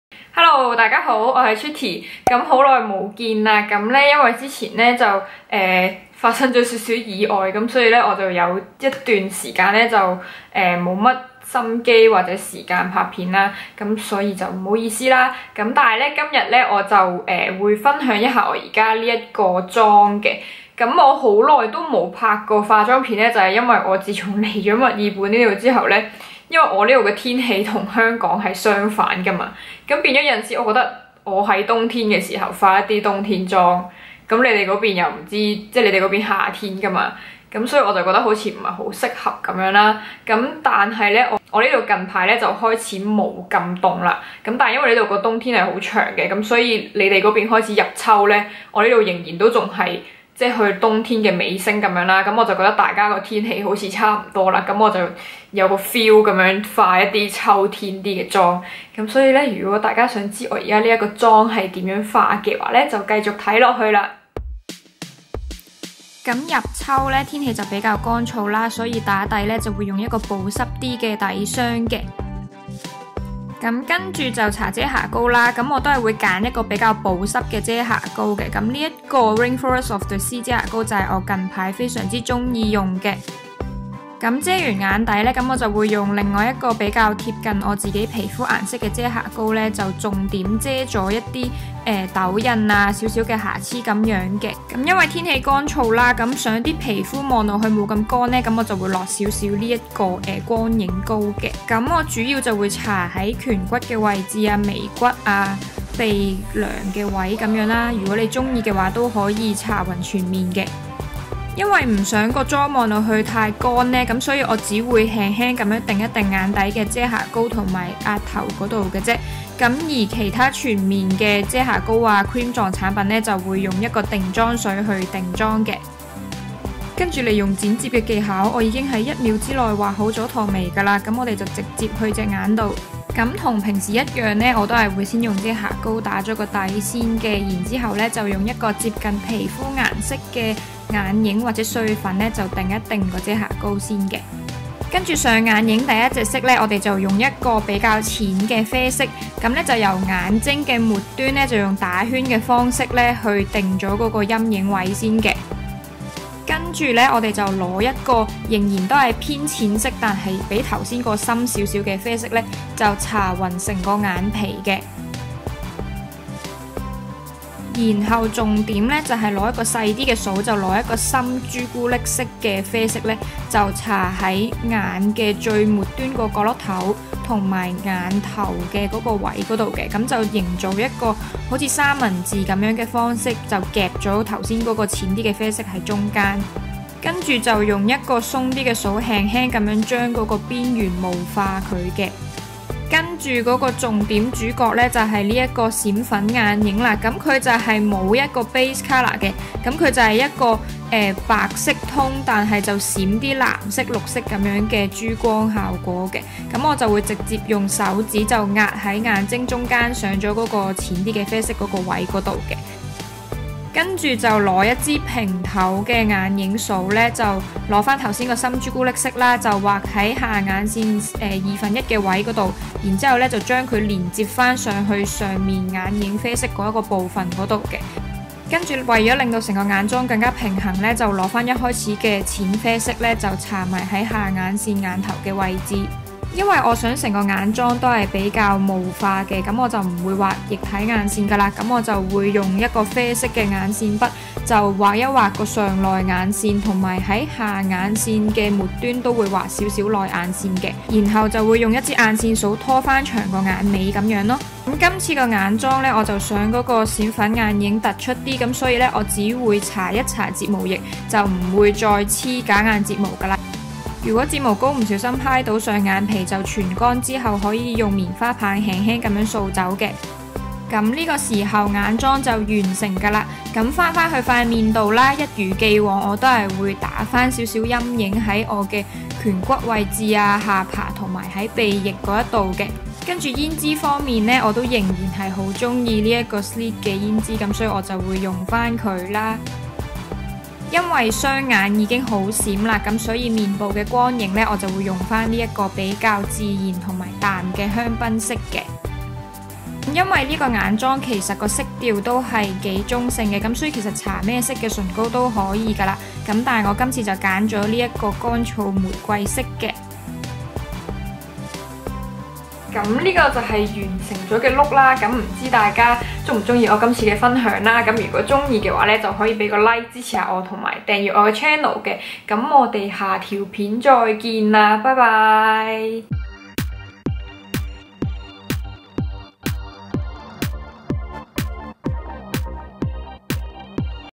秋季閃爍眼妝分享 Fall sparkle eye look*TWEETY*
早幾個星期因為健康問題所以都冇同大家拍片分享，但係我而家返嚟啦！希望大家都會鍾意我今次嘅眼妝分享啦🙂🙂

產品使用:

Haruhada
UV cut gel SPF50+ PA
(桌悦)

Tarte
Rainforest of the sea aquacealer concealer - light neutral

Urban decay
Naked skin weightless complete coverage concealer - light neutral

NYX
Away we glow strobing cream - 02

Kat Von D
Brightening powder - petal

Glam glow
Glowsetter makeup setting spray

Kat Von D
Brow struck dimension powder - dark brown

Becca
Ombré rouge eye palette

Kat Von D
Alchemist holographic transformer face and eye highlight - teal(aquamarine)

Essence
Long lasting eye pencil - 02 hot chocolate

Essence
The false lashes mascara

Charlotte Tilbury
Film star bronze and glow face exult & highlight - fair-medium

Sleek
Blush - Suede 211

Nudestix
Lip + cheek pencil - mystic/whisper